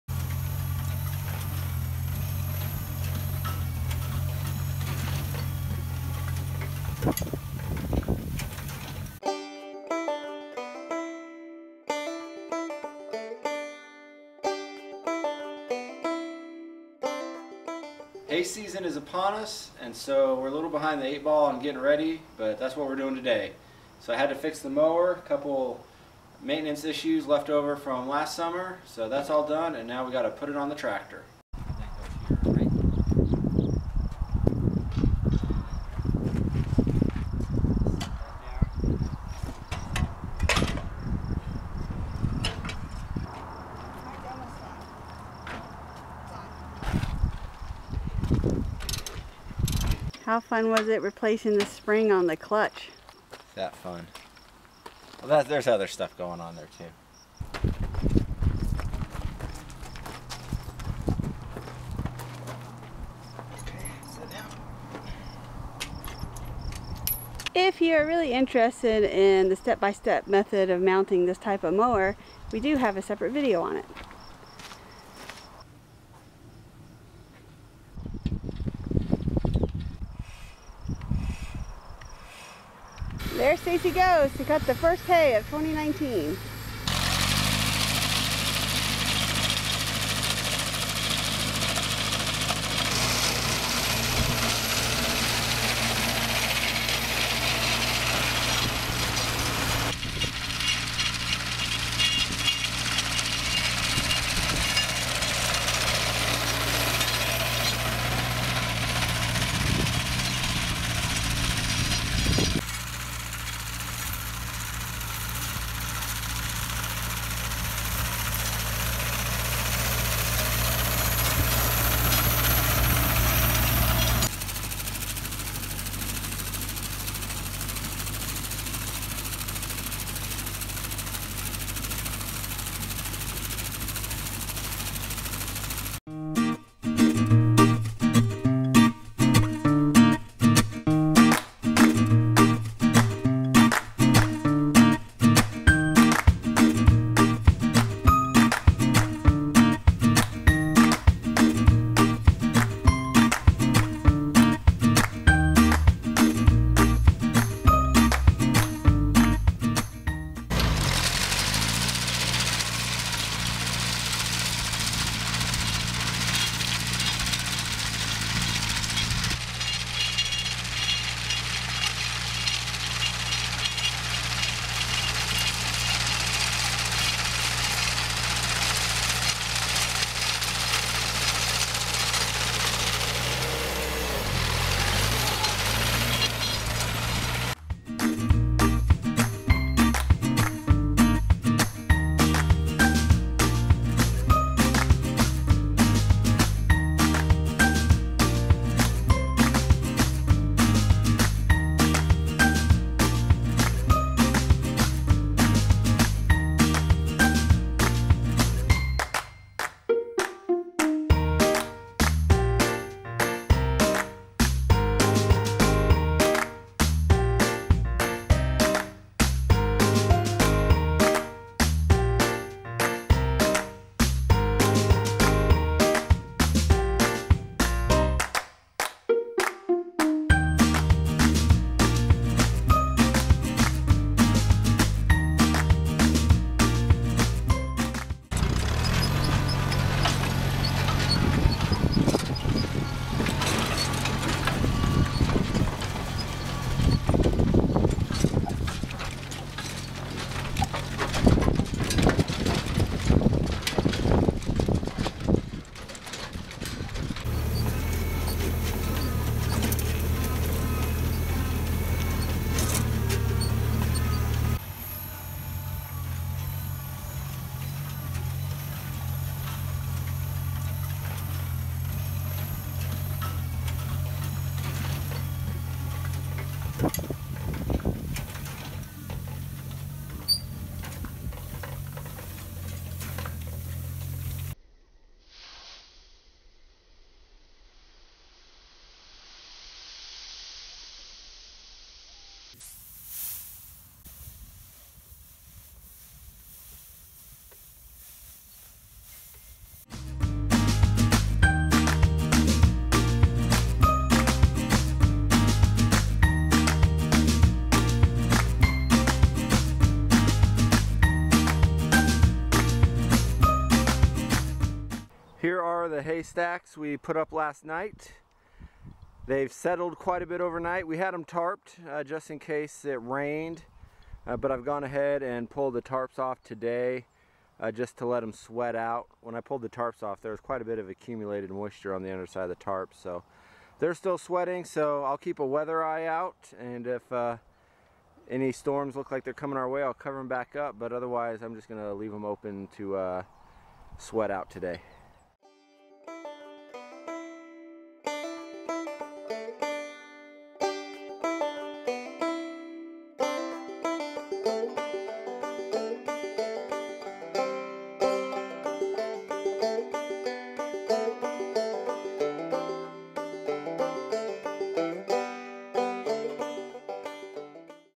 First Loose Hay of 2019
We harvested our first hay this 2019 season! Still using our Farmall Cub to cut and pull the hay wagon; New Holland Workmaster 40 with International Harvester 14 hay rake; and of course the awesome John Deere Hayloader! Since we still need to build our barn, we do have to put up hay outside again this year. Bummer! But we found the "hay muffin" method from last year using two cattle panels in a hoop worked very well! So, we are putting up 4 hay muffins this year, and a hay cake. mmm. cake! This video shows the start of the season with the hay muffins. The hay cake will be another video!

Check out our first loose hay video from 2018, and mounting the 22 Mower video too if you need step by step instructions!